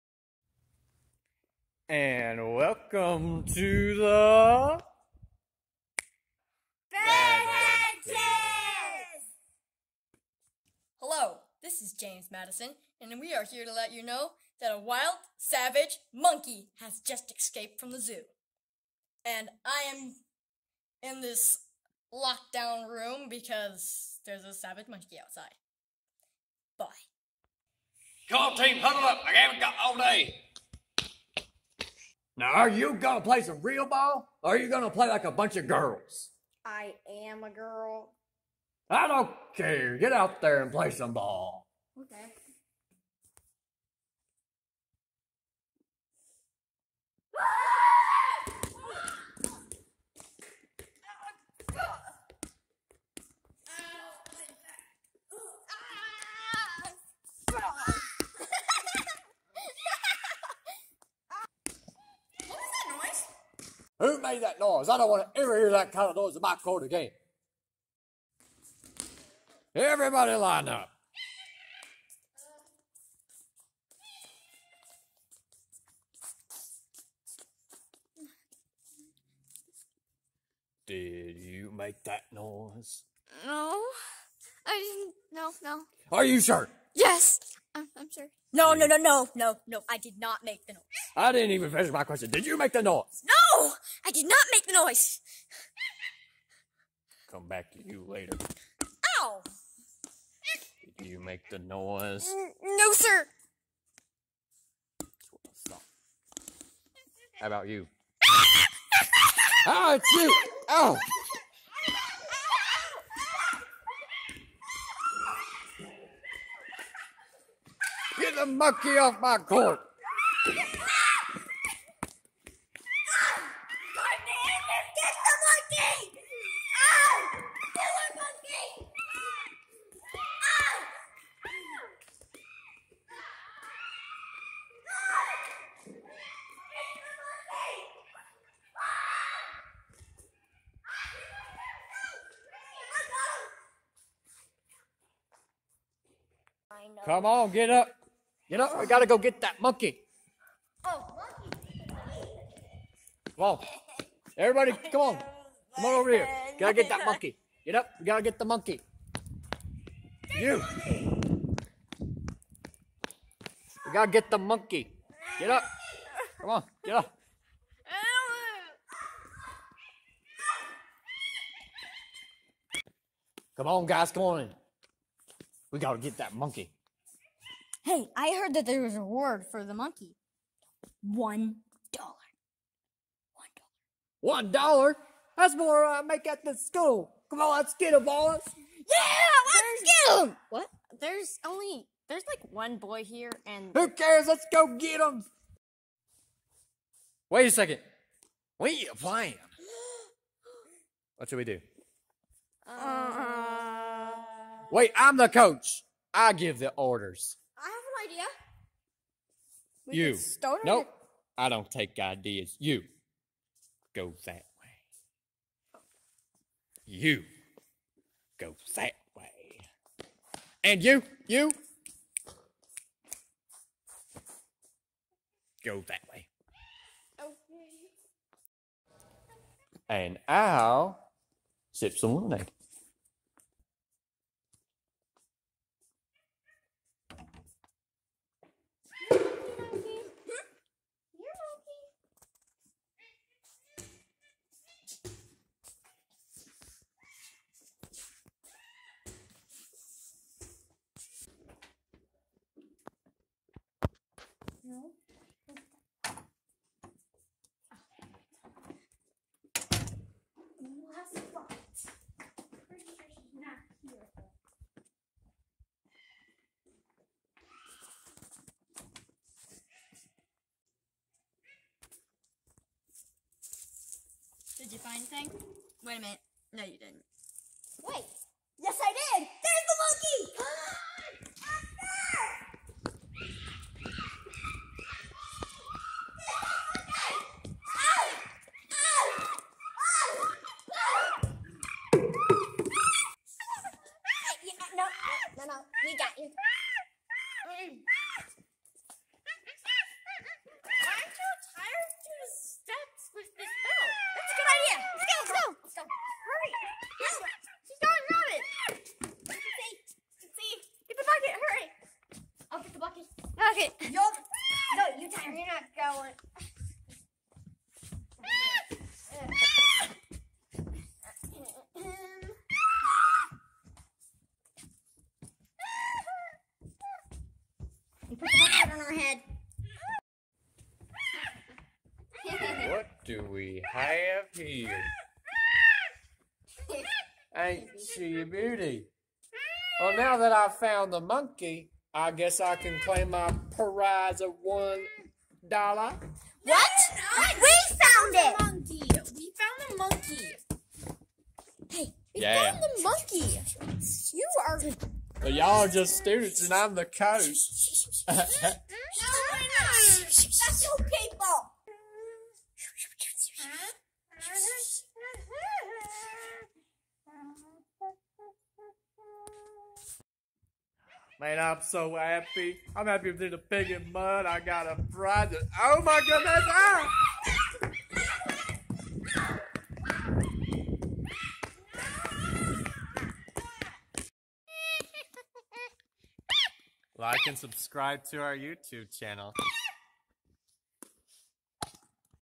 Alabama coach VS Wild Savage Monkey
Coach with a pathetic salary exacts his revenge on a wild monkey.